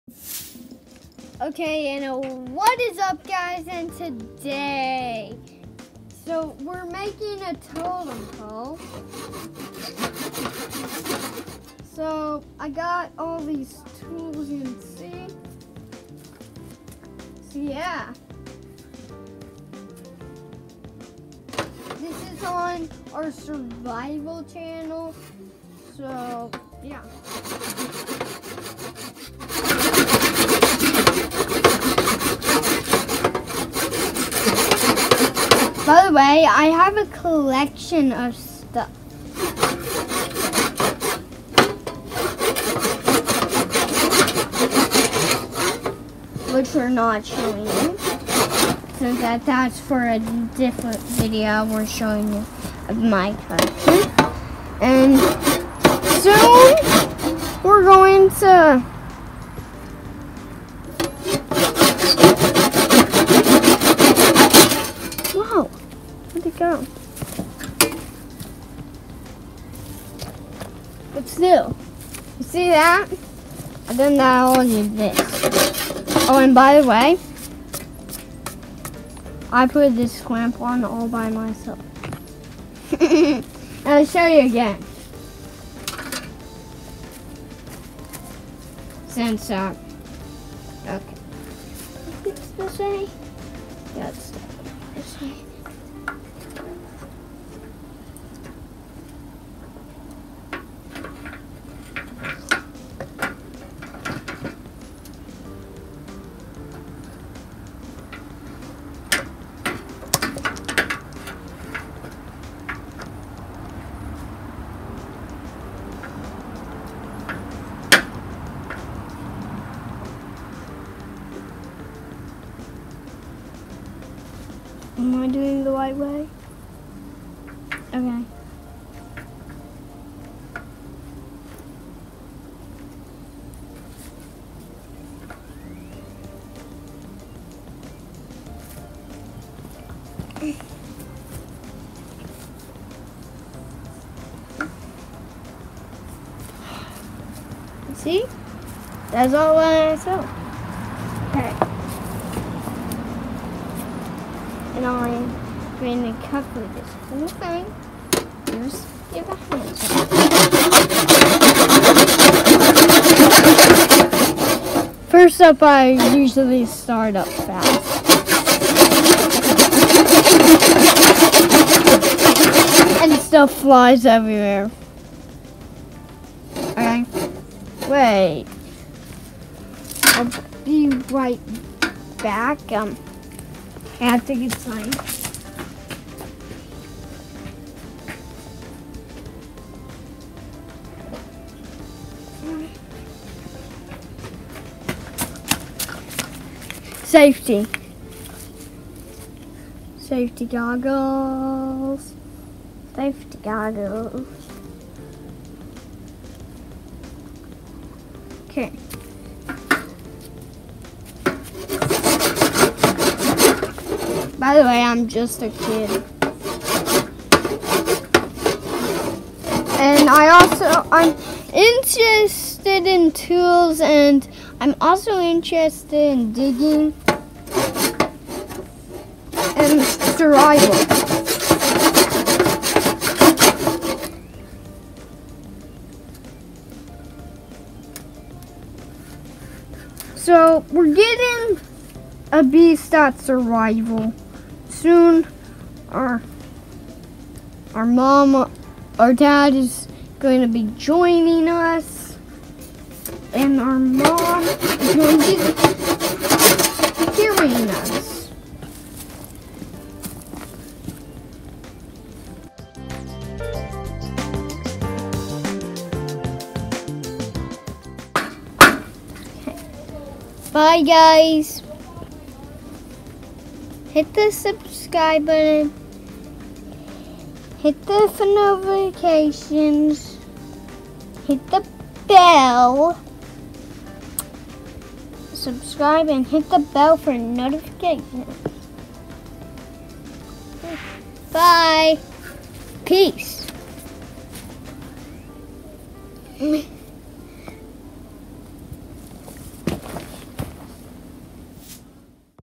Real Life Minecraft Survival ~ Crafting Table
Join VinZ in another episode of Real Life Minecraft!
 Get a tour of his Crafting Table and latest project.